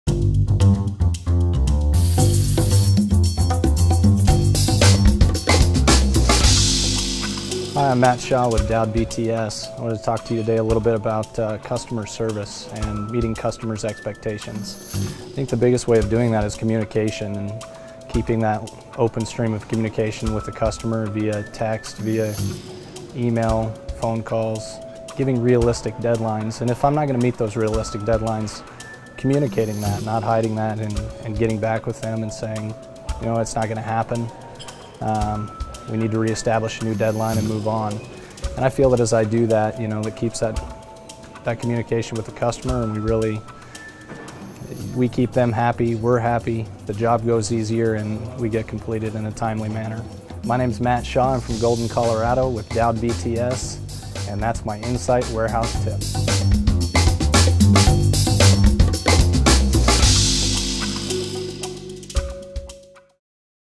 Insight Warehouse Tip with Matt Shaw
What’s the secret to keeping a customer happy when you miss a deadline? Matt Shaw, Design & Project Management at Doud BTS, Inc. in Colorado, gives us his solution.